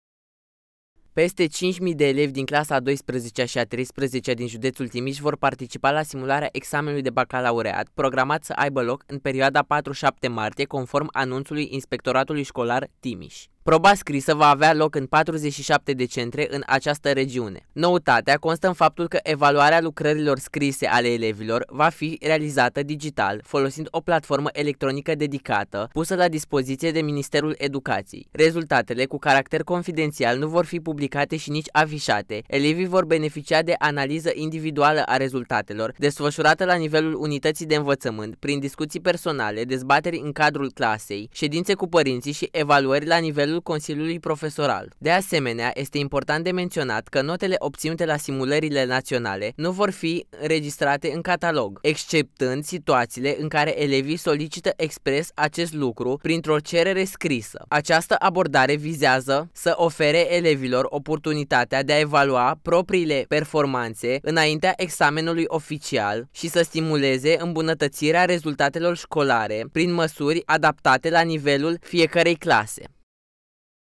5.000 de elevi vor susține simulările de Bacalaureat în luna martie
Peste 5.000 de elevi din clasele a 12-a și a 13-a din județul Timiș vor participa la simularea examenului de Bacalaureat, programată să aibă loc în perioada 4 – 7 martie, conform anunțului Inspectoratului Școlar Timiș. Proba scrisă va avea loc în 47 de centre din această regiune